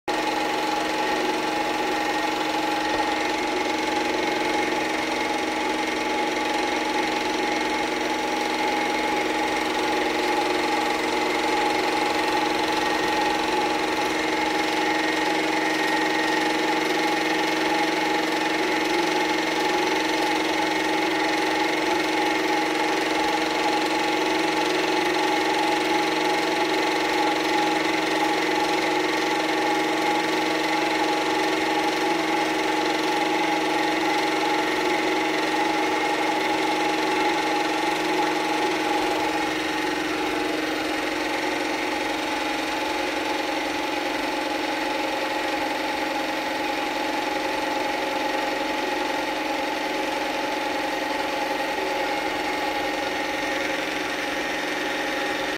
Danfoss PVG32 proportional hydraulic valve test run with self made radio control
Radio receiver and transmitter electronics self made.

Arduino based
My own design of internal pcbs
Åkerströms MC200P radio controller housing with 6x one axis joysticks and custom internal pcb with atmega/arduino based mcu.